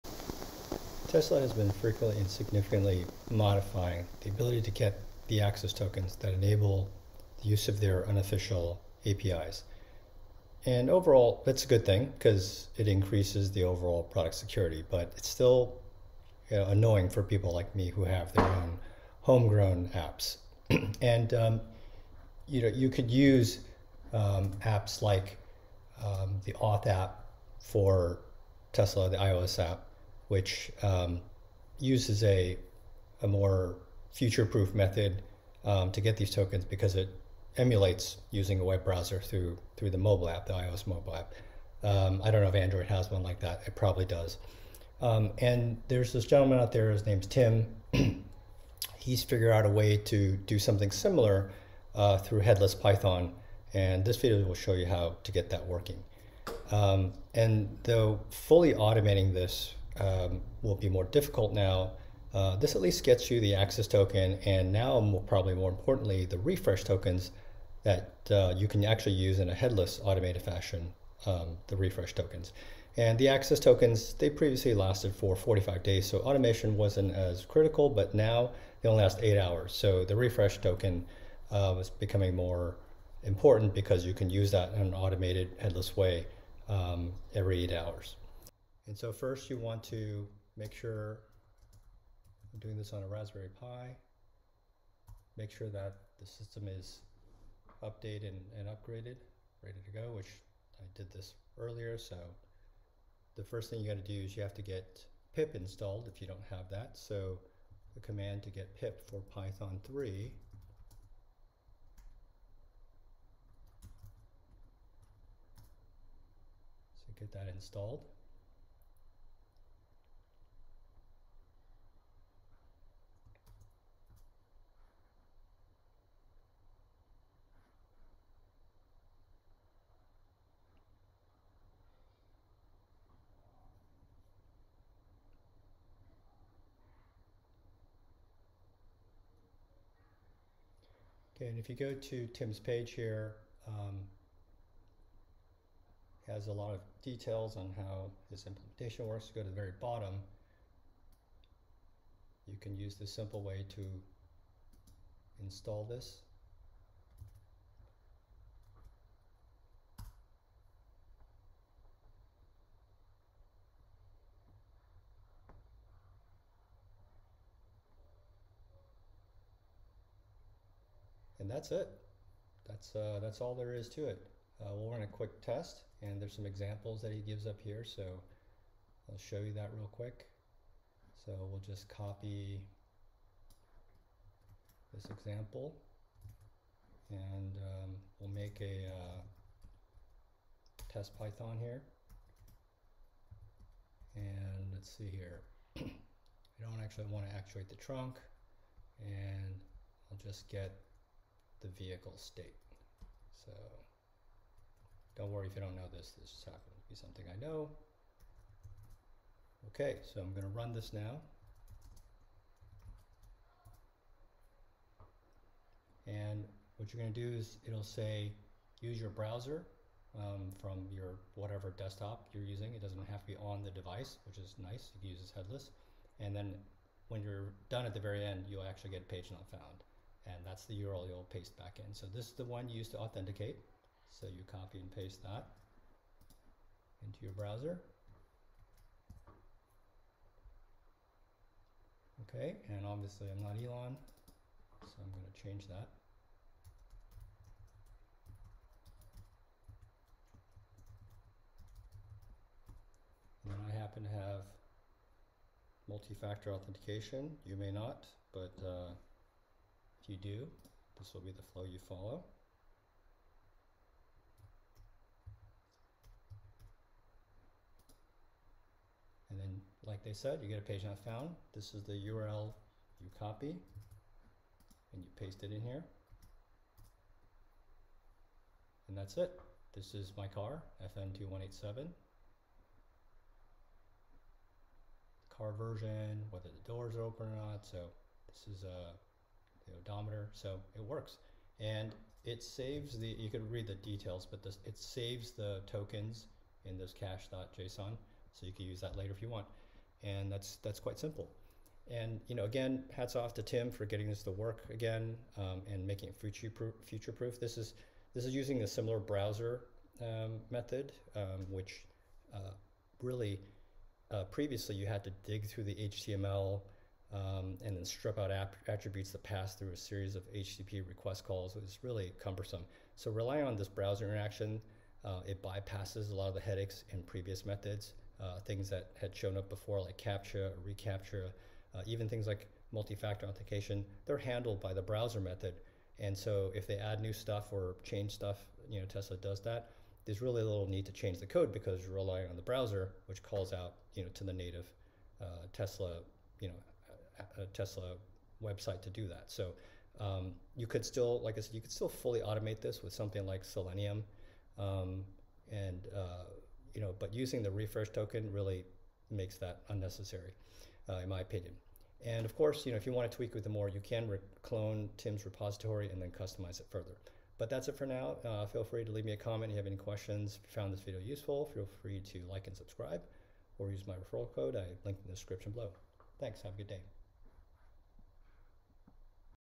Tesla Authentication - Finally Found Something That Works!
After putting off figuring out how to fix my code to keep up with the latest Tesla authentication changes, I’m relying on Tim Dorssers’ implementation to grab the access token in the rare occurrence if my refresh token expires or is invalidated.  I’ll show you how easy and simple he’s made it.

Latest refresh token code on GitHub (the video refers to a deprecated syntax)